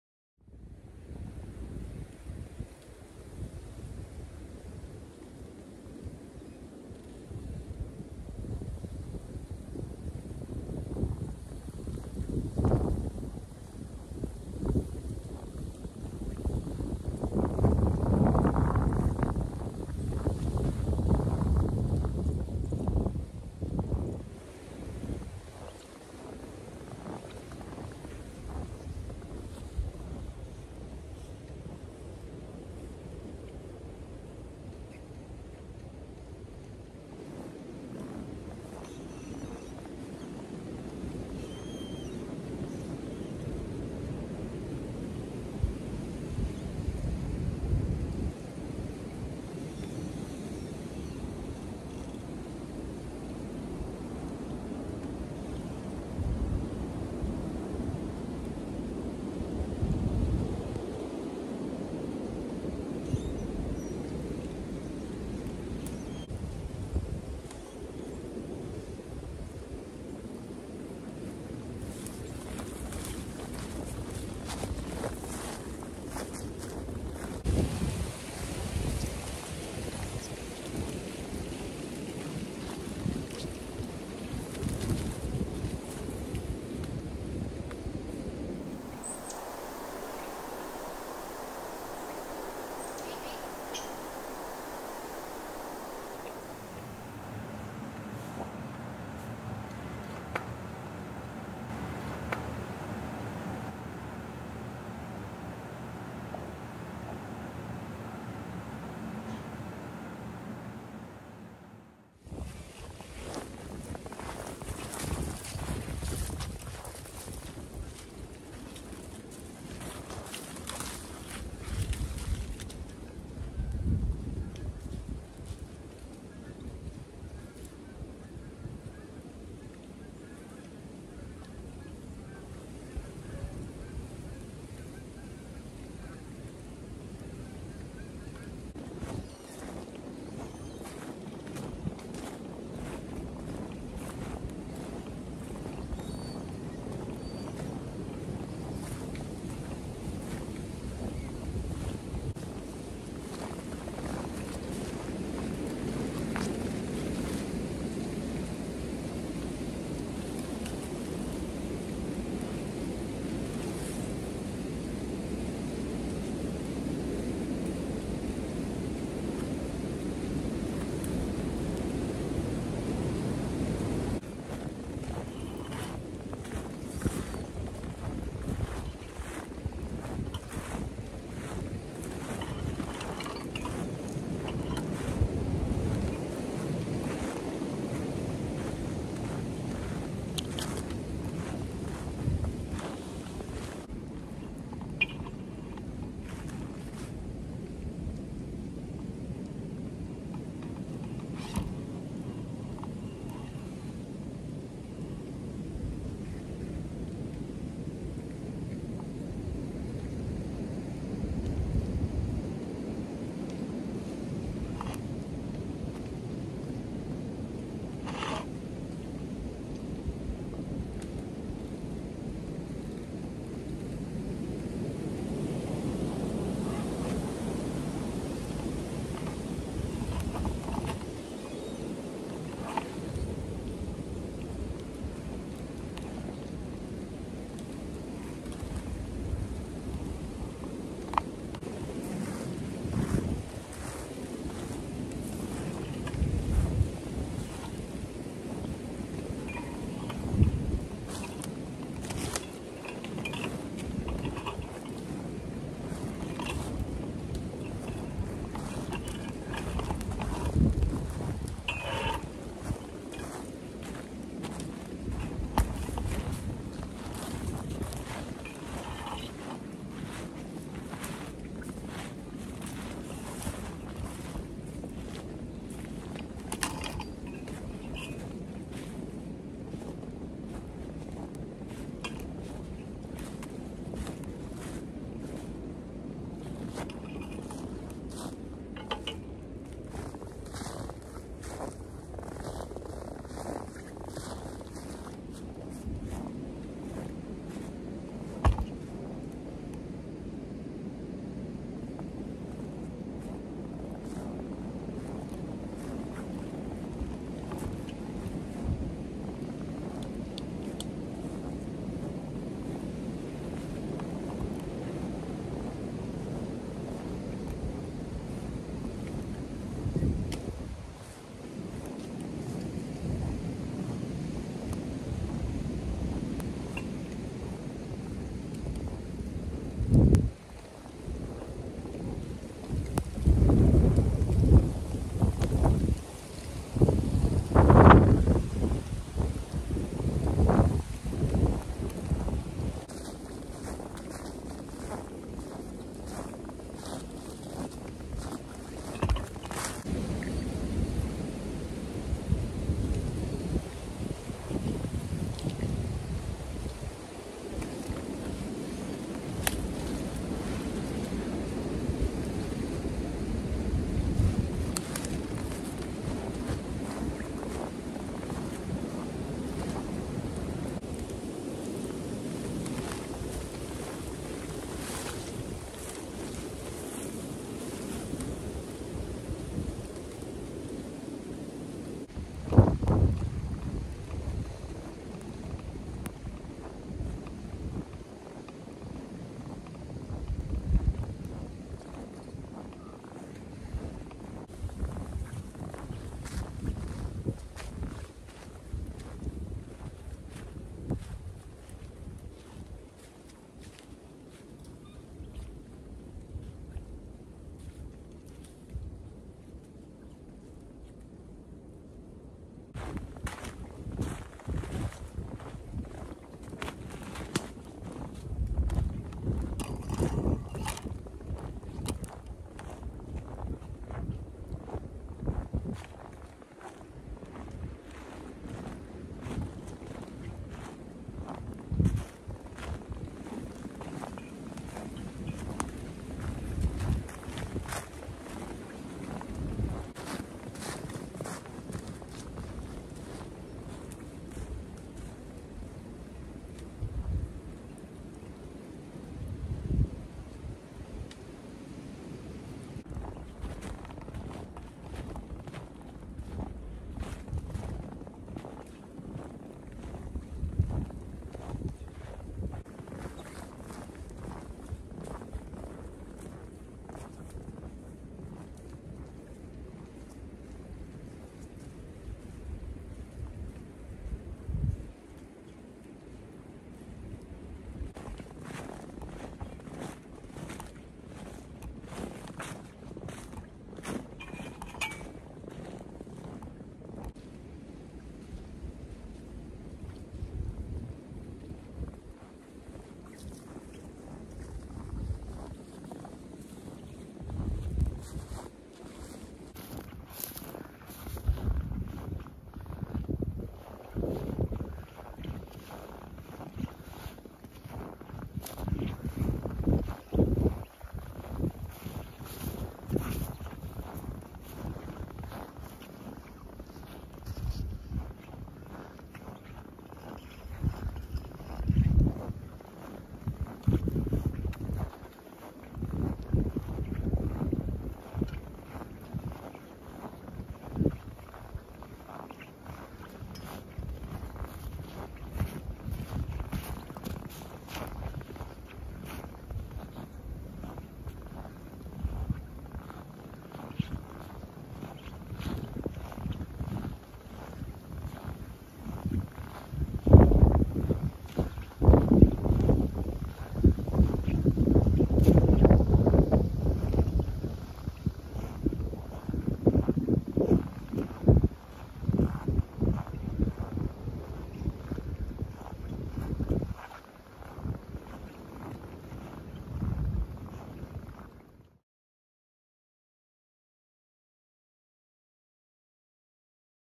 Frozen Woods Walk
Come with me as I walk through the woods and listen to the Blizzard Winds blow and observe woods and nature at her finest.

Winter Blizzard Hike | ASMR | Relaxing Nature Walk | Below Zero | Freezing Cold Snow | Woods Nature.